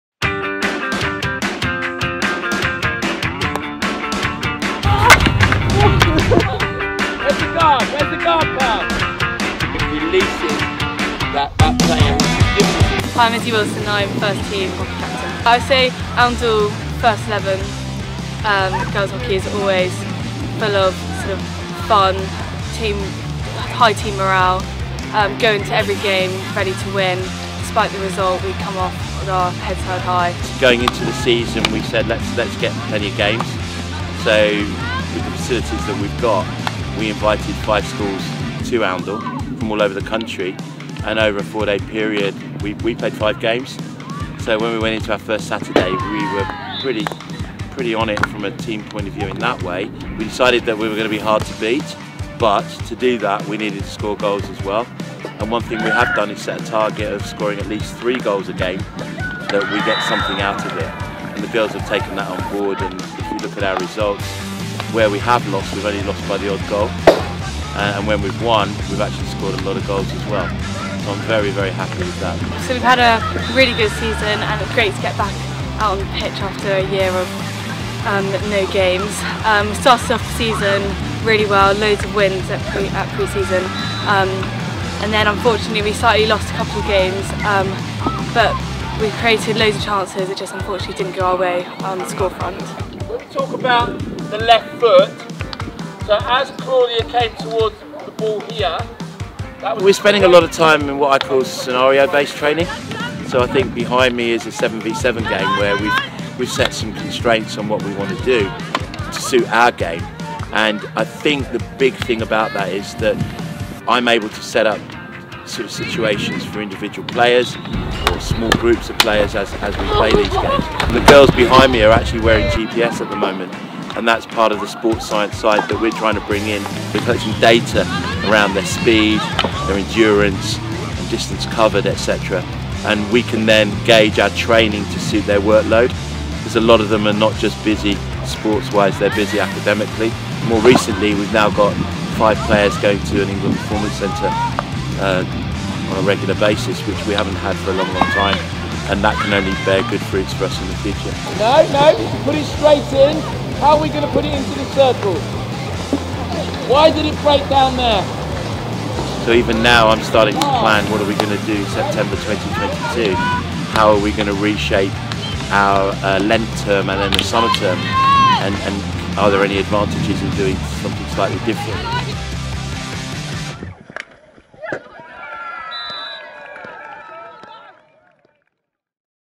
Head of Hockey and 1st XI Girls Hockey Captain review the season | Sport at Oundle School
🏑 Watch as Izzy (Sn U6), 1st XI Girls Hockey captain, and Head of Hockey Grahame Mansell-Grace share their reviews of the season, talk all things training and share exciting updates on sports science and @englandhockey performance centre call-up's.

👏🏼 Well done to all Girls Hockey players across the age groups in the Michaelmas Term as we look forward to Boys Hockey teams gracing the Astro pitches in the Lent Term.